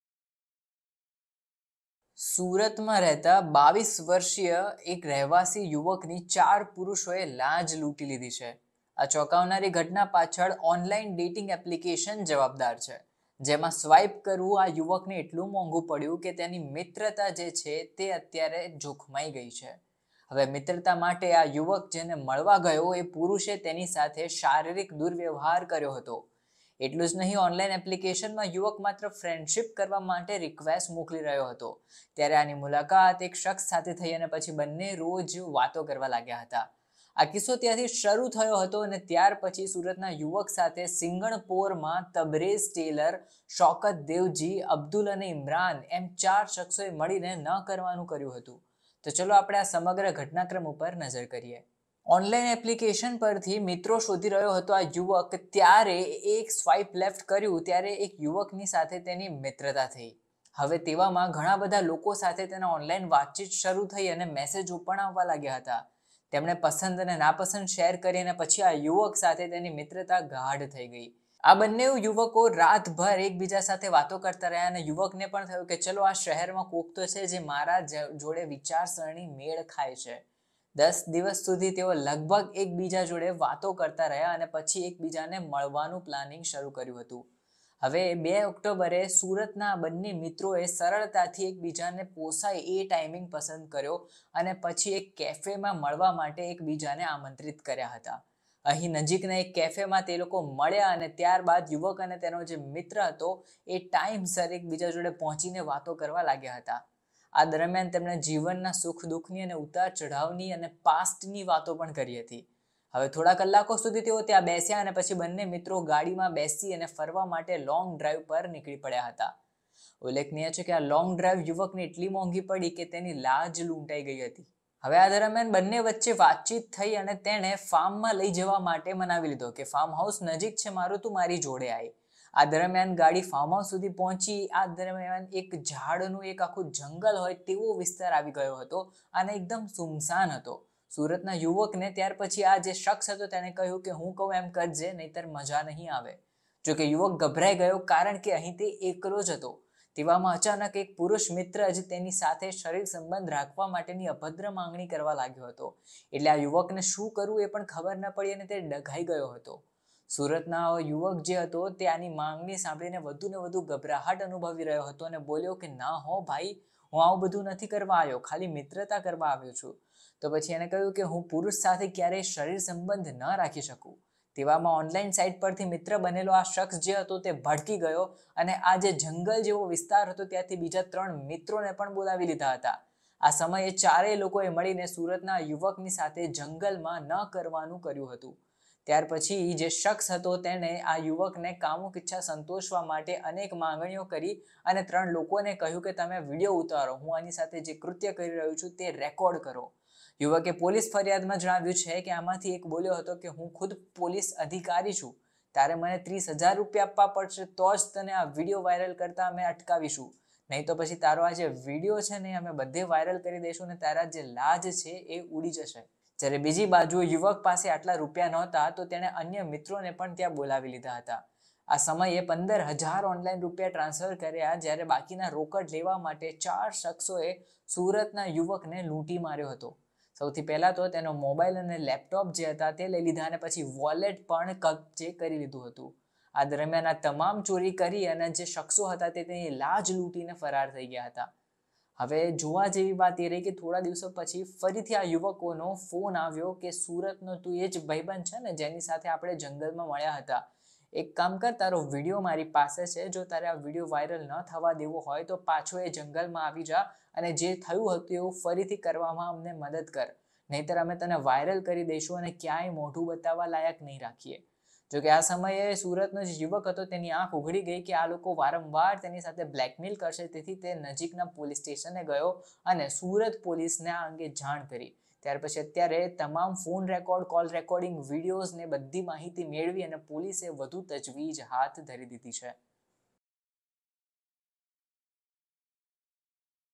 સુરતના યુવકની 4 પુરુષોએ લાજ લૂંટી, ડેટિંગ એપથી ફ્રેન્ડશિપ બાદ જંગલમાં લઈ ગયા ને કાંડ થયો
surat 4 men took youth to jungle: ઓનલાઈ ફ્રેન્ડિશપ એપ પરથી યુવકે મિત્રતા કરી અને ત્યાર પછી સુરતમાં અજાણ્યો પુરુષ મિત્રએ તેનો વિશ્વાસ જીતી બધી હદો વટાવી નાખી હતી.